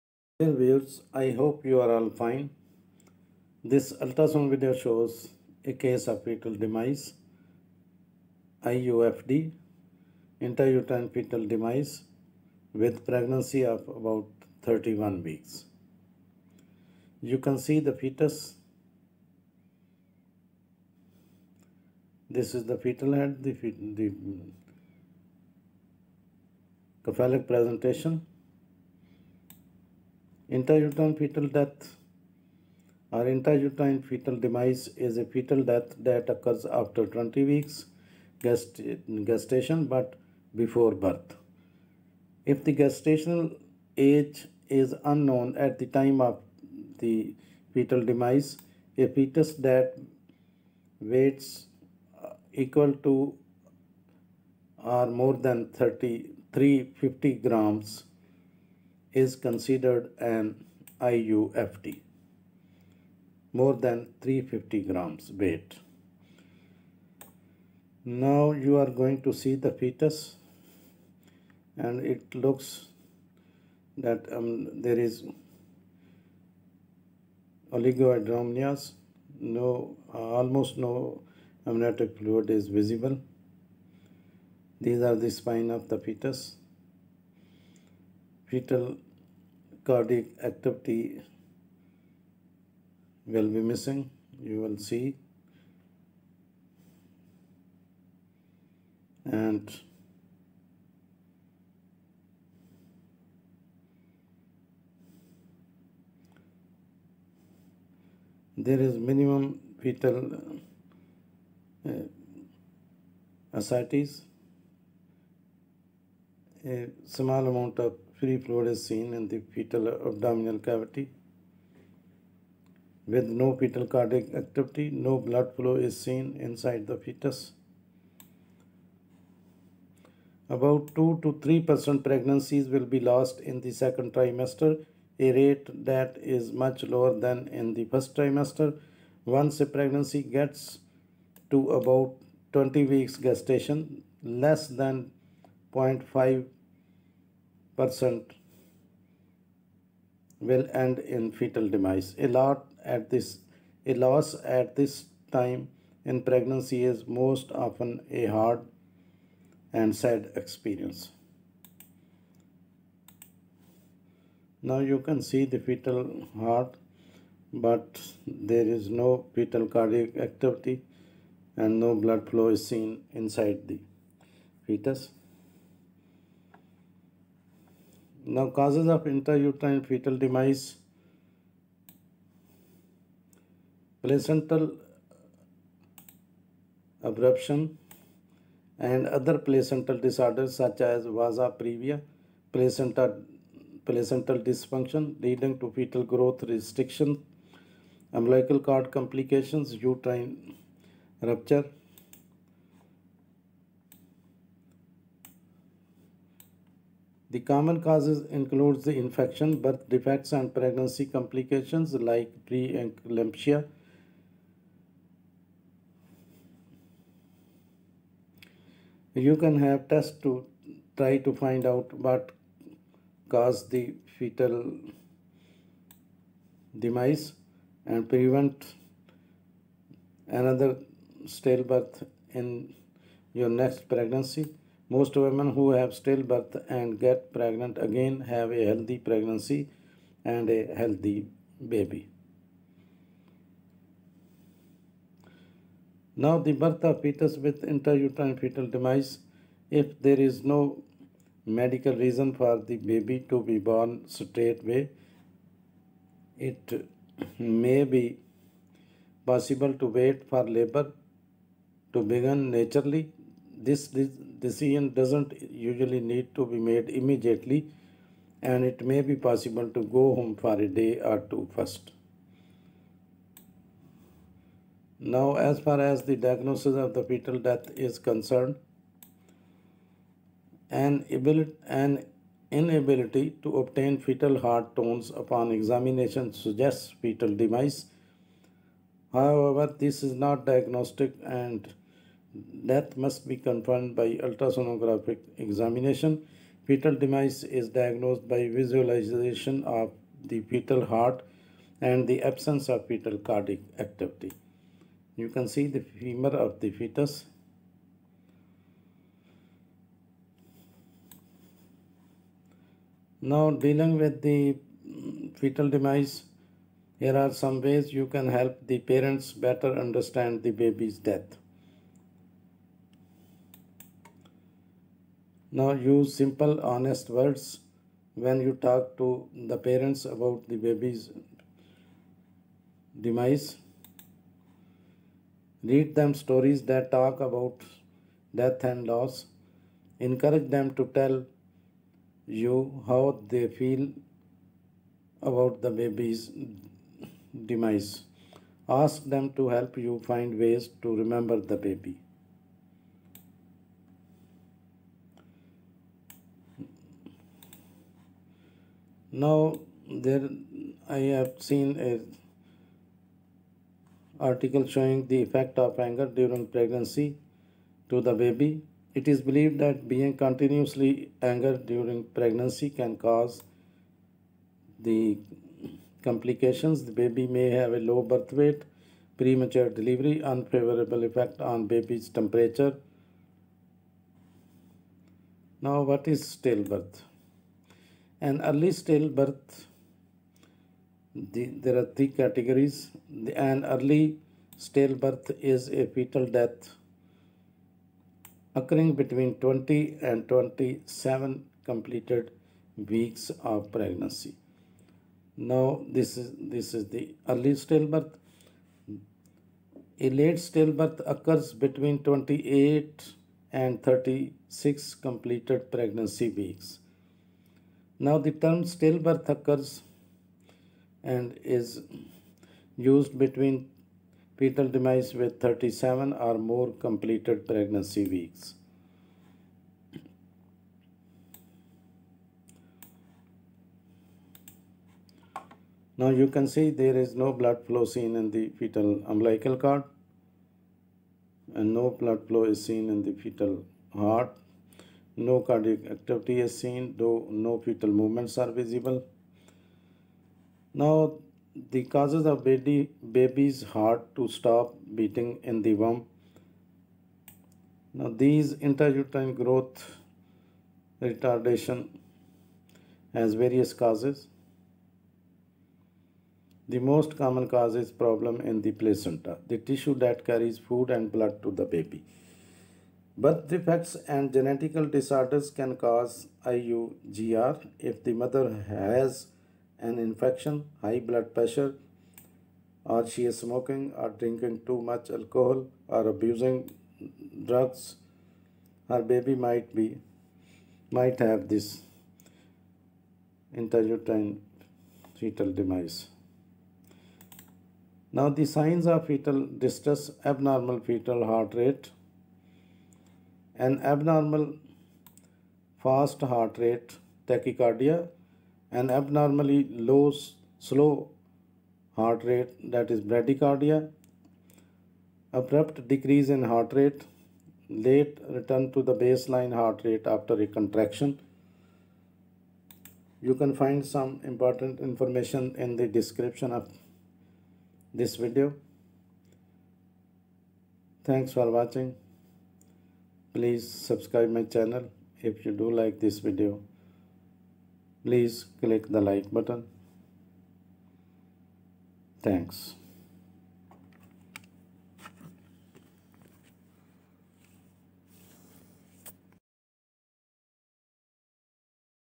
Ultrasound Video showing Fetal demise ( IUFD ) with a Pregnancy of about 31 weeks.
This video shows Fetal demise ( IUFD ) with a Pregnancy of about 31 weeks.
Intrauterine fetal demise (IUFD) is fetal death that occurs after 20 weeks of gestation but before birth. If the gestational age is unknown at the time of death, a fetus that weighs equal to and more than 350 g is considered an IUFD.
About 2-3% of pregnancies will be lost in the second trimester, a rate that is much lower than in the first trimester. Once a pregnancy gets to about 20 weeks gestation, less than 0.5% will end in fetal demise. A loss at this time in pregnancy is most often a hard and sad experience.
                      Causes intrauterine fetal demise
Placental abruption and other placental disorders (such as vasa previa) Placental dysfunction leading to fetal growth restriction. Umbilical cord complications. Uterine rupture.
Common causes include infections, birth defects, and pregnancy complications, like preeclampsia. You can have tests to try to find out what caused your baby's death and prevent another stillbirth in your next pregnancy. Most women who have a stillbirth and get pregnant again have a healthy pregnancy and a healthy baby.
              Birth of fetus with Intrauterine fetal demise
If there's no medical reason for the baby to be born straight away, it may be possible to wait for labor to begin naturally. This decision doesn't usually need to be made immediately, and it may be possible to go home for a day or two first.
                     Diagnosis of Fetal Death
An inability to obtain fetal heart tones upon examination suggests fetal demise; however, this is not diagnostic and death must be confirmed by ultrasonographic examination. Fetal demise is diagnosed by visualization of the fetal heart and the absence of cardiac activity.
                                 Dealing with fetal demise
Here are some ways you can help the parents better understand the baby's death:
Use simple, honest words when you talk to them about the baby's death. ...
Read them stories that talk about death and loss. ...
Encourage them to tell you how they feel about the baby's death.
Ask them to help you find ways to remember the baby.
                   Effect of anger during pregnancy to the baby
It is believed that being continually angry during pregnancy can cause the following complications: The baby may have a low birth weight. Premature delivery. Unfavorable effect on the baby's temperament (more prone to irritability and depression).
                                 What is Stillbirth?
An early stillbirth is a fetal death occurring between 20 and 27 completed weeks of pregnancy.
A late stillbirth occurs between 28 and 36 completed pregnancy weeks.
A term stillbirth occurs between 37 or more completed pregnancy weeks.
        Causes of the baby's heart to stop beating in the womb
IUGR has various causes. The most common cause is a problem in the placenta (the tissue that carries food and blood to the baby). Birth defects and genetic disorders can cause IUGR. If the mother has an infection, high blood pressure, is smoking, or drinking too much alcohol or abusing drugs, her baby might have IUGR.
                   Signs of fetal distress: abnormal fetal heart rate
1). An abnormally fast heart rate (tachycardia)
2). An abnormally slow heart rate (bradycardia)
3). Abrupt decreases in heart rate (variable decelerations)
4). Late returns to the baseline heart rate after a contraction (late decelerations).
    Tricks for Getting Your Baby to Move in Utero
1. Have a snack.
2. Do some jumping jacks, then sit down.
3. Gently poke or jiggle your baby bump.
4. Shine a flashlight on your tummy.
6. Lie down.
7. Talk to baby.
8. Do something that makes you nervous (within reason).